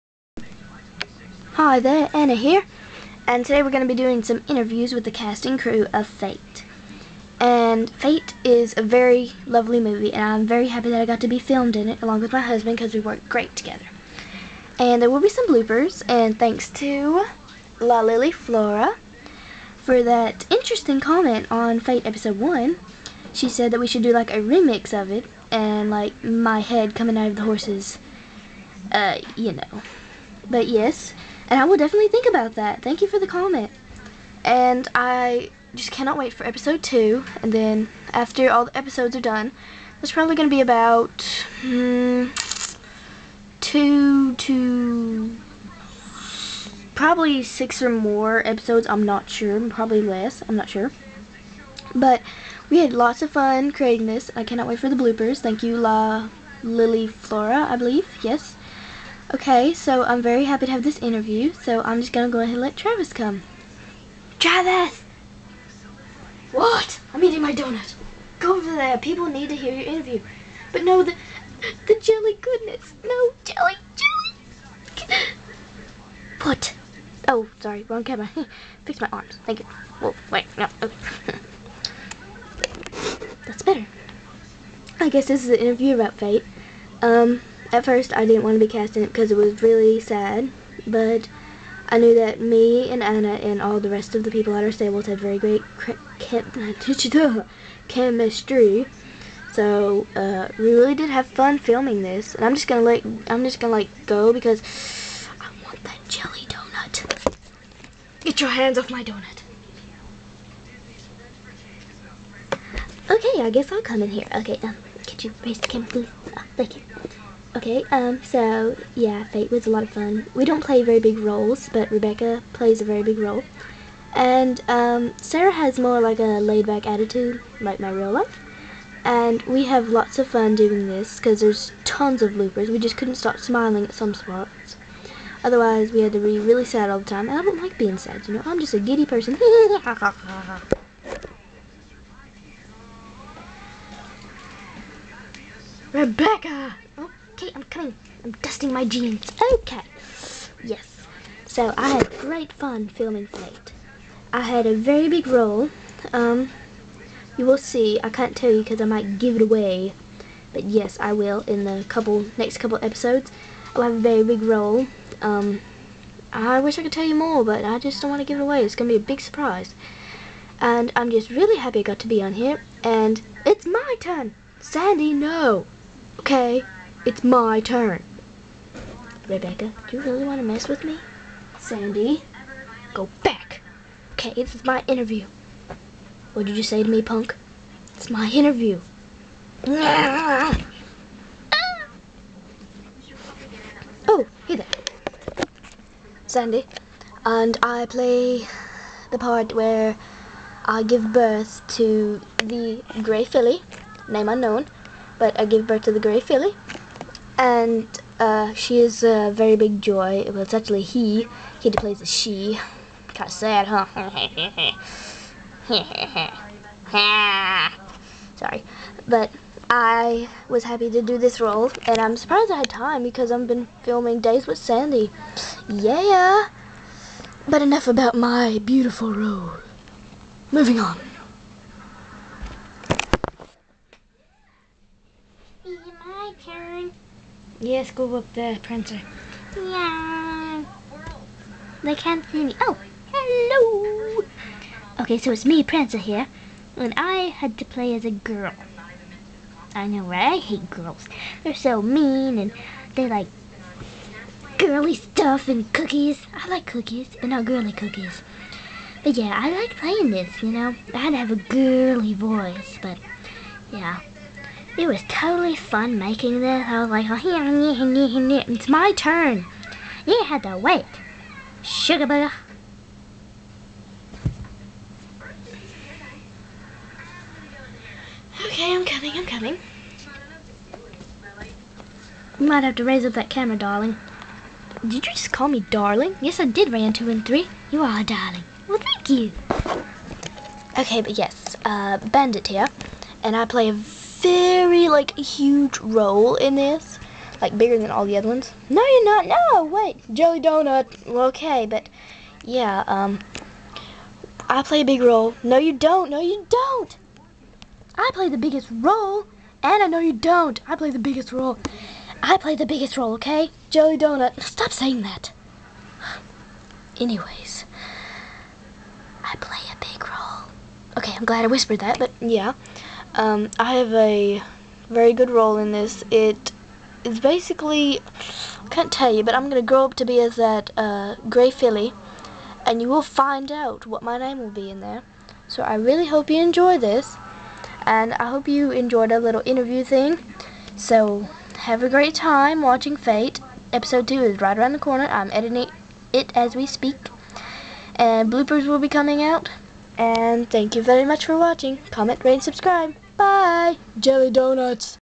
Fate "Interviews" (My Breyer Horse series/movie)
I think Travis likes Jelly doughnuts TOO MUCH! O.O XD
Enjoy the interviews!! -starts editing ep.2-

RayAnn